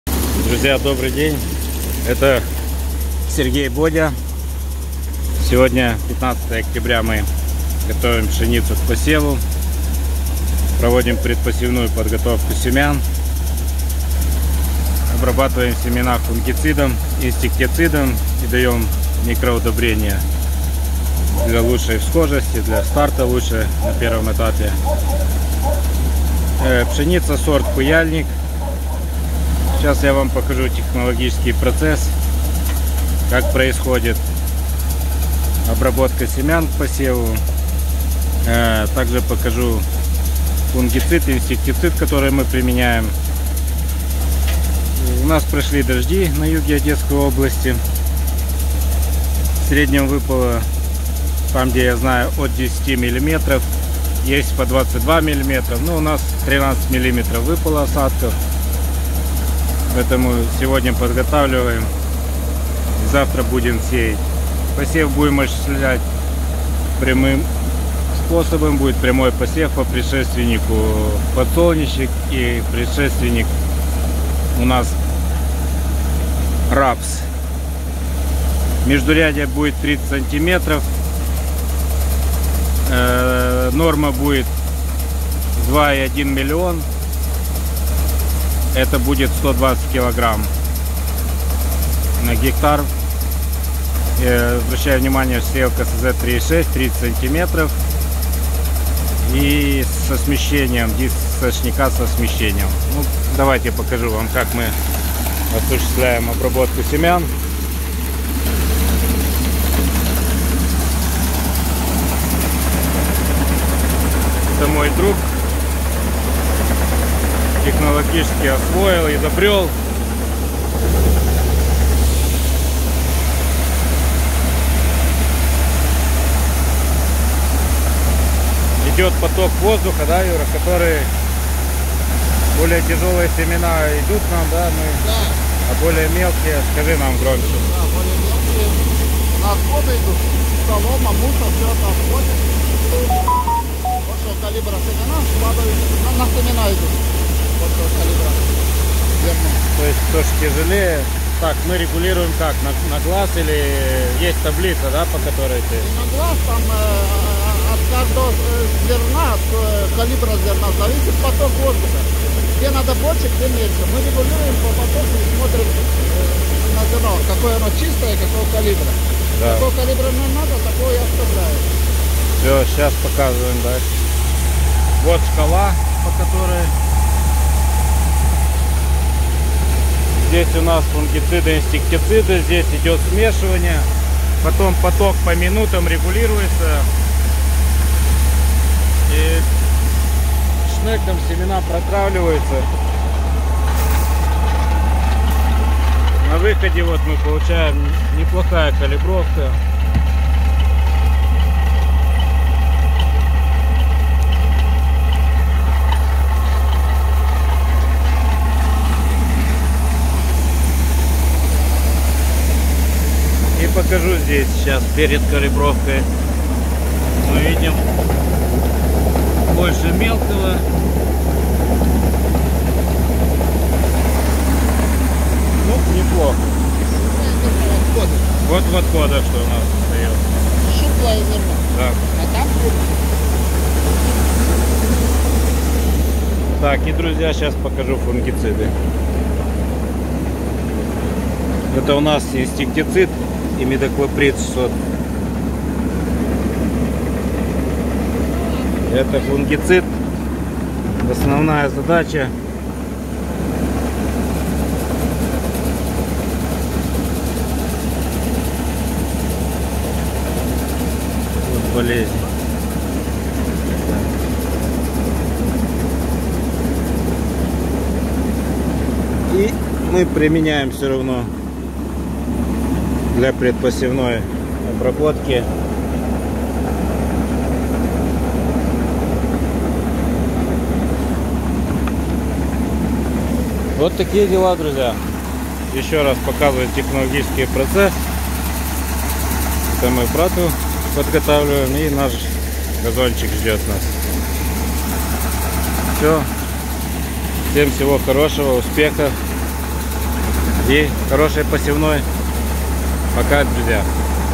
Подготовка семян к посеву.Технологический процесс.Озимая пшеница.
ПОДГОТОВКА семян к посеву.Озимая пшеница,фунгицид,инстектицид,аминокислоты.Зона степи Украины.